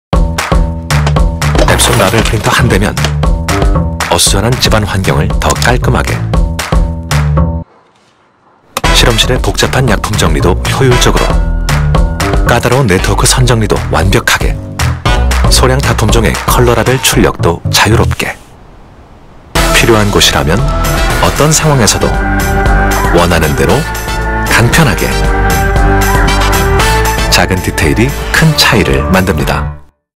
2020 엡손 It’s in the Details 캠페인 | 라벨프린터 편
[캠페인 배경]
1942년 탄생한 엡손은 오래된 시계구조에서 시작되었습니다. 정밀한 시계 기술에서 출발한 엡손은 현재 전세계 8만면을 거느린 글로벌기업이 되었습니다. 세상의 모든 차이는 정밀한 기술로부터 시작된다는 철학을 바탕으로 [ 작은 디테일이 큰 차이를 만든다 = It’s in the Details ] 캠페인을 전개하고 있습니다. 본 캠페인은 엡손의 주요 핵심 카테고리인 프린터, 프로젝터, 산업용 로봇, 스마트글래스 등 4가지 품목의 멀티소재 광고로 엡손의 브랜드 철학을 고객에게 알리고 있습니다.

[캠페인 실행 및 결과]
엡손 브랜드캠페인의 핵심타겟은 B2B 비즈니스고객이 타겟이나 B2B에 한정하지 않고 B2C고객을 아우르는 크리에이티브로 고객들에 어필하고 있습니다. 유튜브는 각 품목 4편 및 종합편 모두 100만이상의 조회수를 기록하였고 목표 KPI를 상회하는 성과를 내었습니다. 특히 글로벌 엡손에서 진행한 캠페인 중 조회수 기준 3위에 랭크되어 내부적으로 좋은 평가를 받고 있습니다.